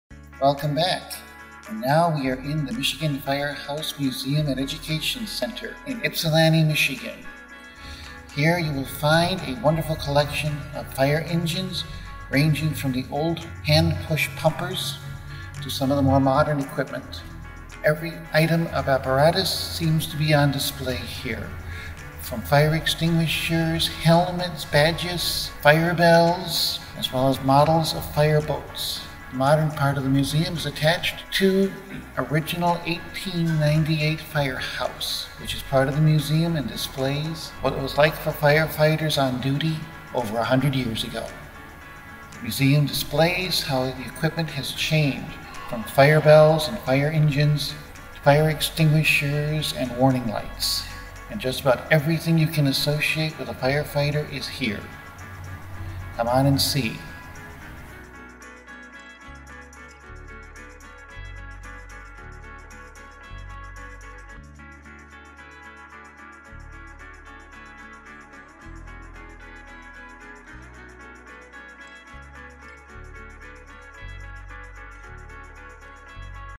Tour the Michigan Firehouse Museum with James Mann
The second installment of museum tours with local historian, James Mann.

Love this video? Check out James' tour of the Yankee Air Museum!